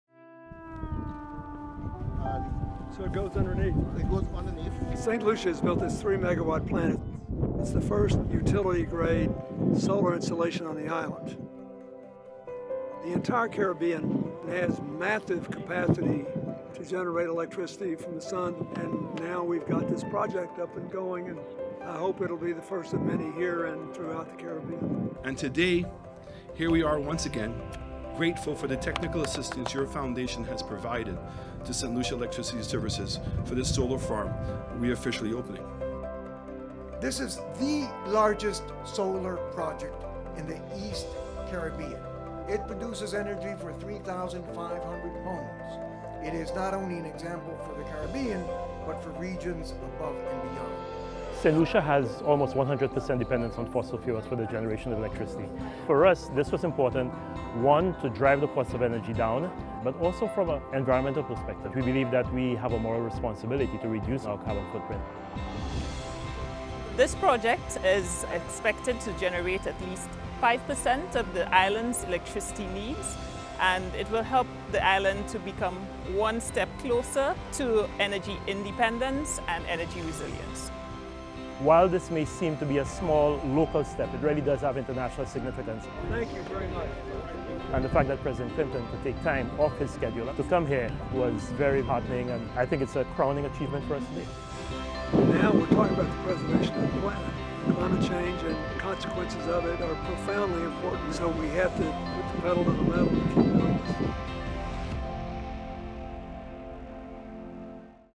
OnAssignment with President Clinton: St. Lucia solar farm
Get a behind-the-scenes look as President Bill Clinton helps open a new 3 megawatt (MW) solar farm of the Saint Lucia Electricity Services Limited (LUCELEC) in La Tourney, Vieux Fort. The utility-scale renewable energy project, the first of its kind in Saint Lucia, was made possible through technical assistance from the Rocky Mountain Institute (RMI) and the Clinton Climate Initiative, an initiative of the Clinton Foundation, which have received financial support from the United Nations Development Programme, the Global Environment Facility, and the Norwegian Agency for Development Cooperation.